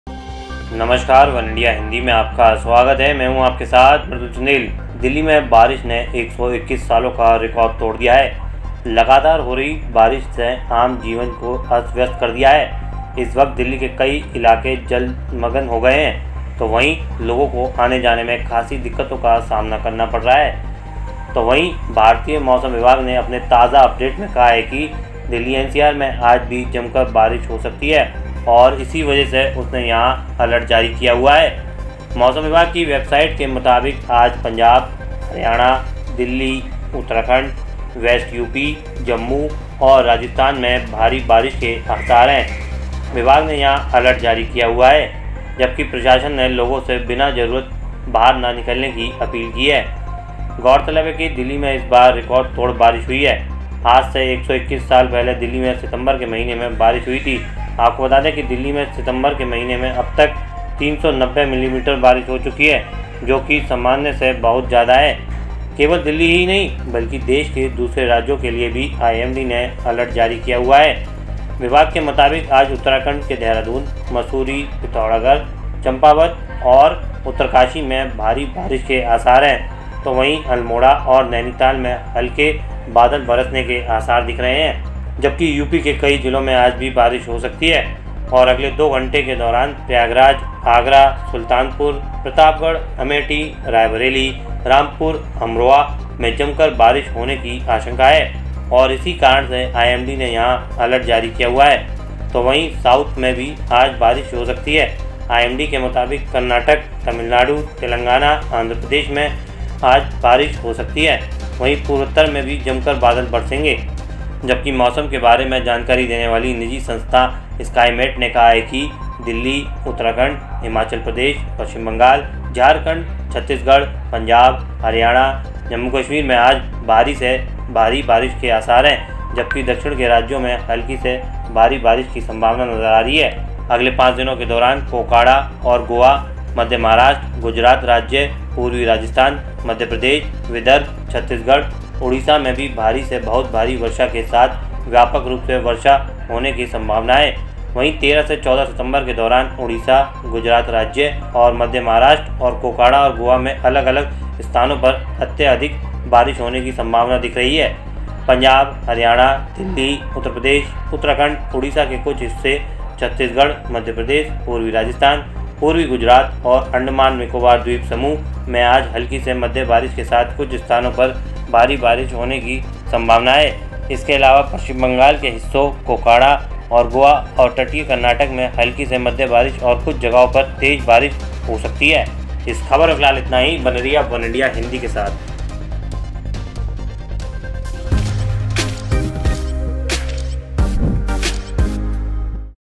Weather Update: Delhi-NCR में आज होगी बारिश, IMD ने इन राज्यों में जारी किया अलर्ट | वनइंडिया हिंदी
Rain in Delhi has broken the record of 121 years. Normal life has been disturbed due to incessant rains. At present, many areas of Delhi have been submerged, while people are facing a lot of difficulties in commuting, while the Indian Meteorological Department has said in its latest update that Delhi-NCR may receive heavy rain today. And that's why he has issued an alert here.

दिल्ली में बारिश ने 121 सालों का रिकॉर्ड तोड़ दिया है। लगातार हो रही बारिश से आम जनजीवन अस्त-व्यस्त हो गया है। इस वक्त दिल्ली के कई इलाके जलमग्न हो गए हैं तो वहीं लोगों को आने-जाने में खासी दिक्कतों का सामना करना पड़ रहा है तो वहीं भारतीय मौसम विभाग ने अपने ताजा अपडेट में कहा है कि दिल्ली-एनसीआर में आज भी जमकर बारिश हो सकती है और इसी वजह से उसने यहां अलर्ट जारी किया हुआ है